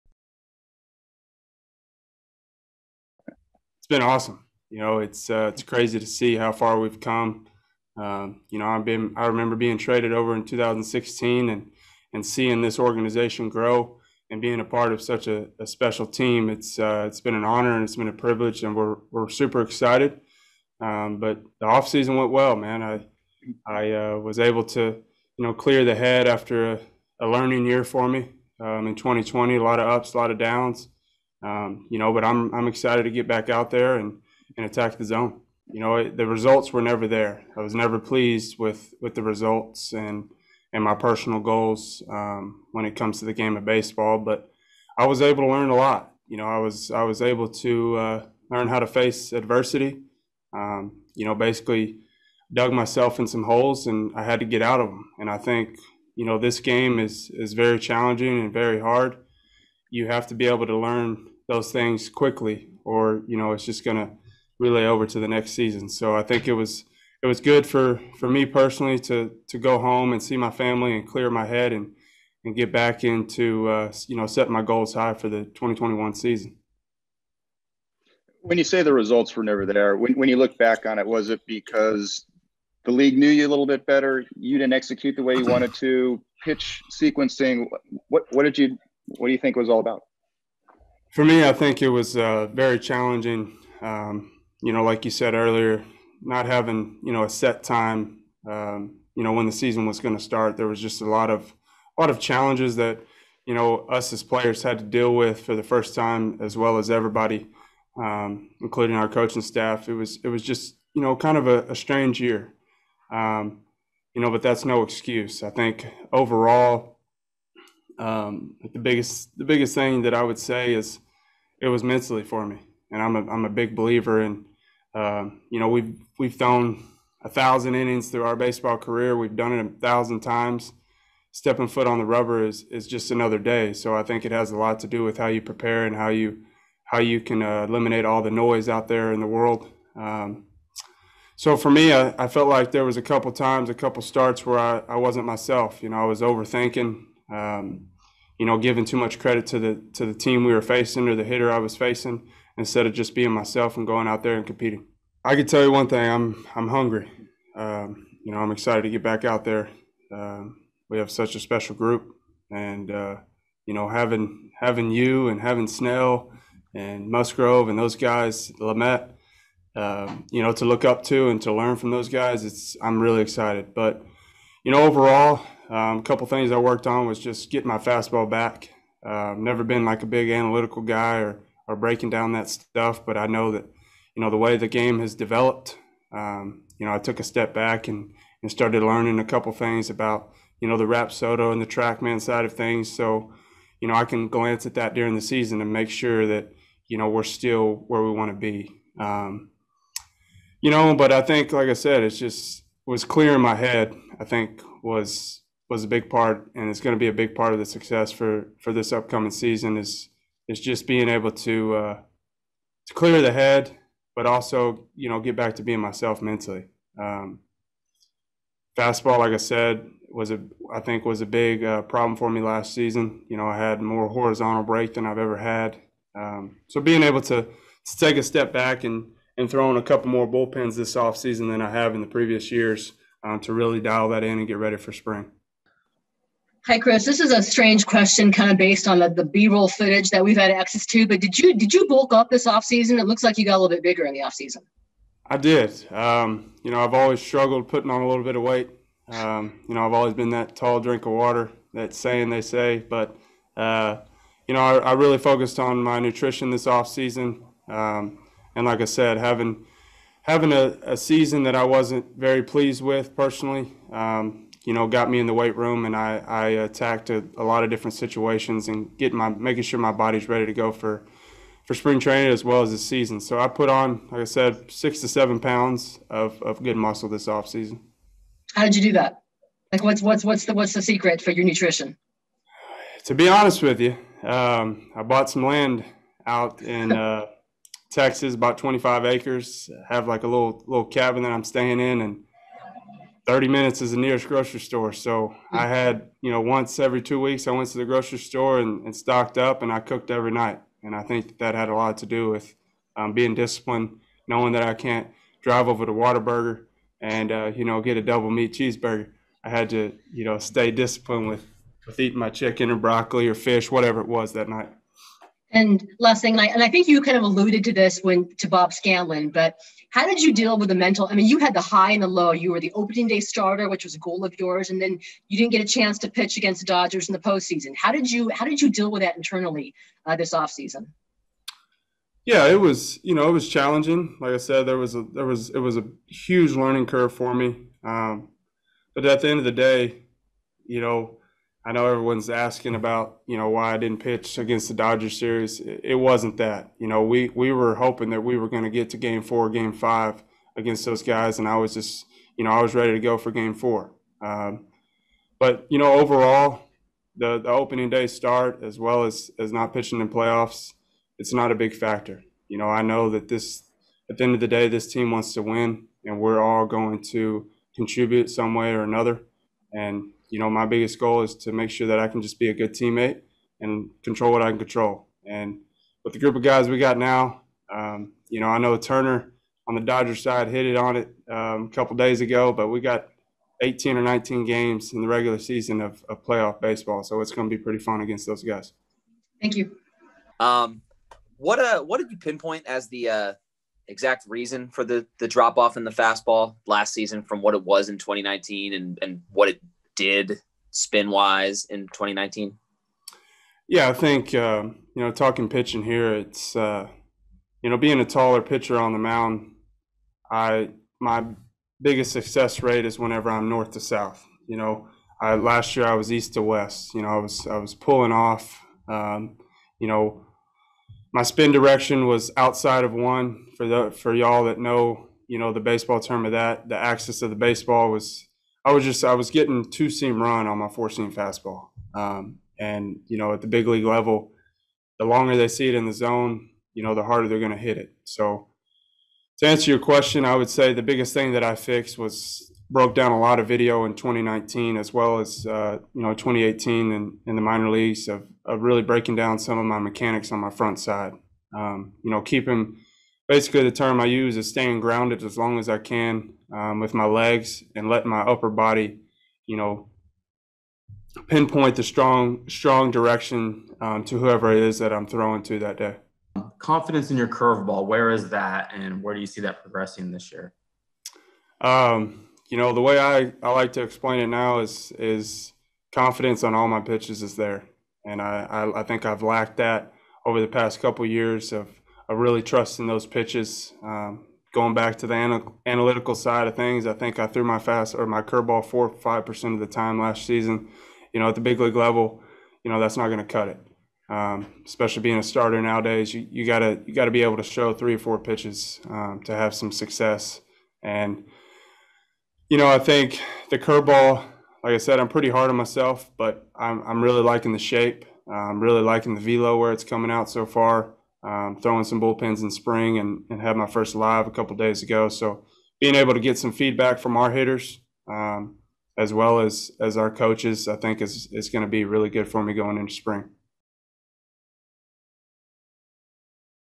Chris Paddack on mental challenges of 2020, confidence in his curveball & why he's hyped for 2021
Padres pitcher Chris Paddack discussed the takeaways from a challenging 2020 season, if he felt hitters adjusted to him, confidence in his curveball. how he bulked up in the off-season and why he's excited about this 2021 team.